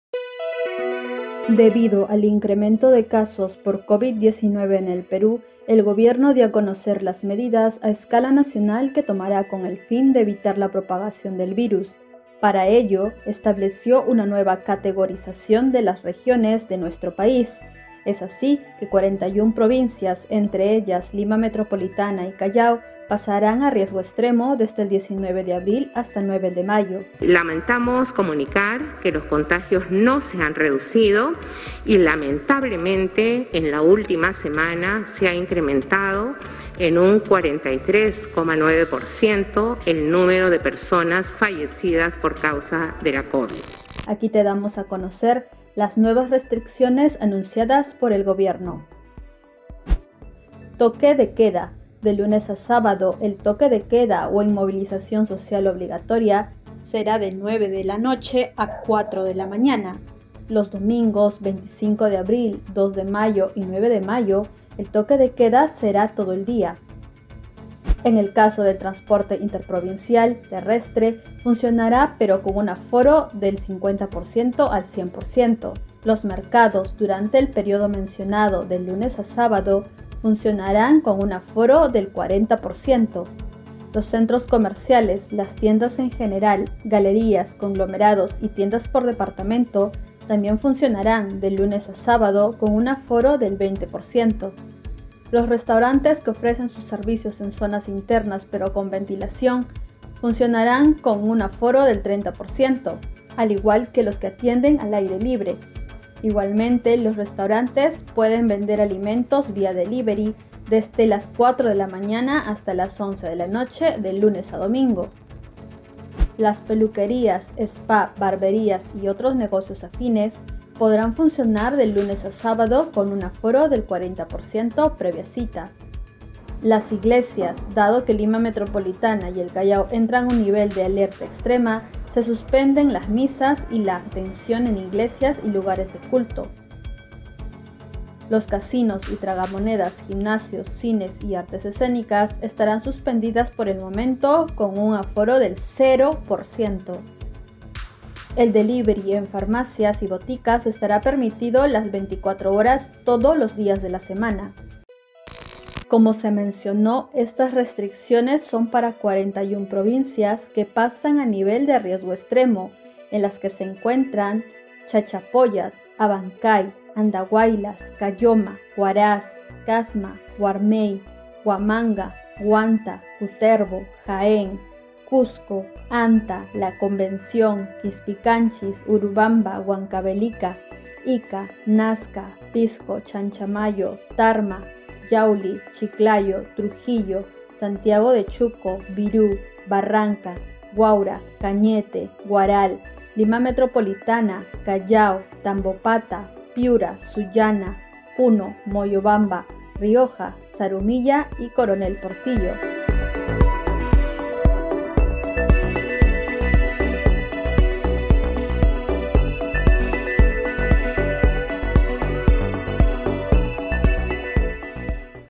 Coronavirus Perú: Las nuevas medidas según nivel de alerta que rigen desde el 19 de abril
Desde el lunes 19 de abril hasta el domingo 9 de mayo, regirán las nuevas restricciones del estado de emergencia nacional frente al COVID-19.Cabe indicar que las medidas serán según la clasificación de provincias de acuerdo al riesgo de contagio de coronavirus: extremo, muy alto y alto.